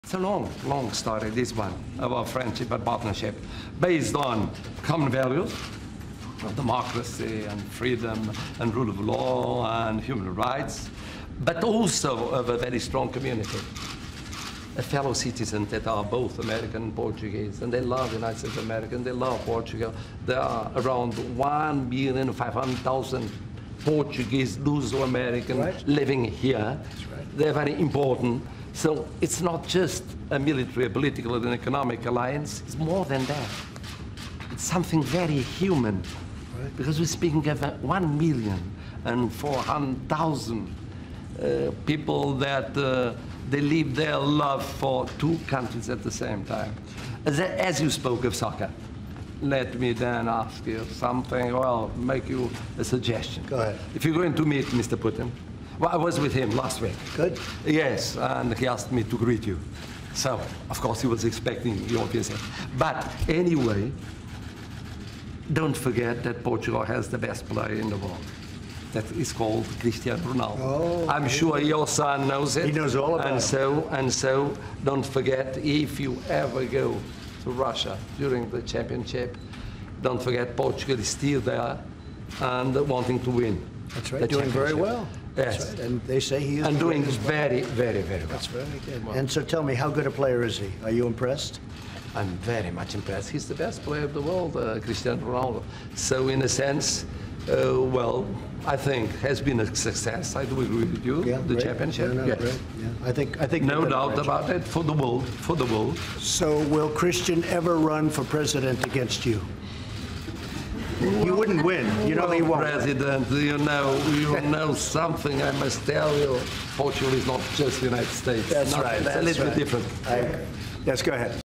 Marcelo Rebelo de Sousa: "Portugal não é os Estados Unidos" | Marcelo e Donald Trump | RTP
O chefe de Estado português trocou alguns comentários sobre o Campeonato do Mundo de Futebol com o seu homólogo norte-americano, Donald Trump, e deu-lhe cumprimentos da parte do Presidente da Rússia, Vladimir Putin.

No início de um encontro com o Presidente dos Estados Unidos da América, Donald Trump, na Sala Oval da Casa Branca, em Washington, em declarações conjuntas à comunicação social, Marcelo Rebelo de Sousa disse-lhe: "Vai encontrar-se com o senhor Putin, bem, eu estive com ele na semana passada e ele pediu-me que lhe enviasse cumprimentos".

"Não se esqueça que Portugal tem o melhor jogador do mundo, que se chama Cristiano Ronaldo. Estou certo de que o seu filho sabe", prosseguiu o Presidente português.

Trump, que antes tinha elogiado o Campeonato do Mundo de Futebol que decorre na Rússia, considerando que "tem sido fantástico", respondeu-lhe que o seu filho "sabe tudo sobre isso".